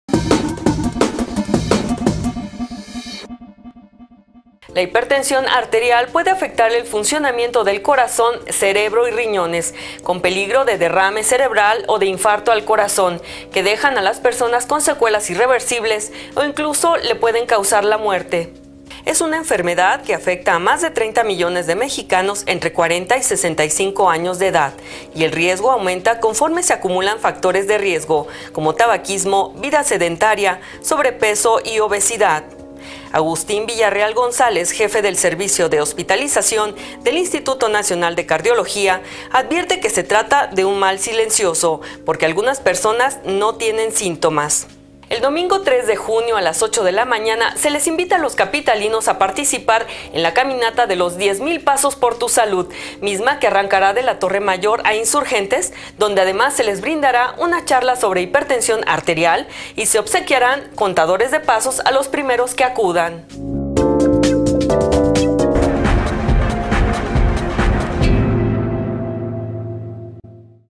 Vértigo Saludable. Hipertensión arterial
La hipertensión arterial puede afectar el funcionamiento del corazón, cerebro y riñones, con peligro de derrame cerebral o de infarto al corazón, que dejan a la persona con secuelas irreversibles o incluso le causan la muerte